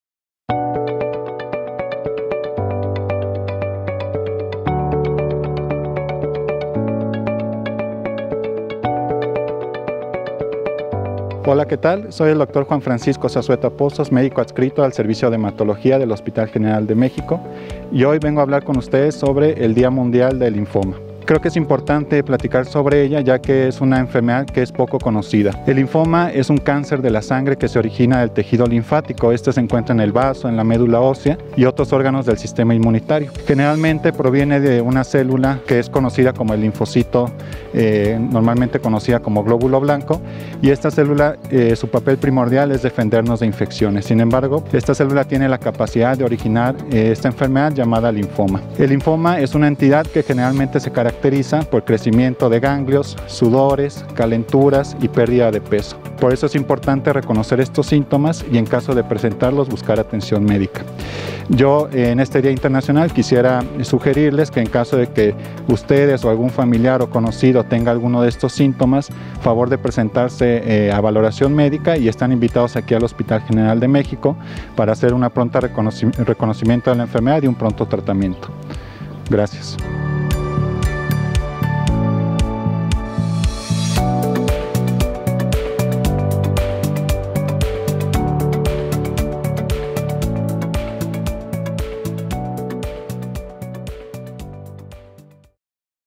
Día Mundial del Linfoma | 15 de Septiembre
Hoy 15 de septiembre se conmemora el Día Mundial del Linfoma y el Dr. Juan Francisco Zazueta Pozos nos brinda esta información.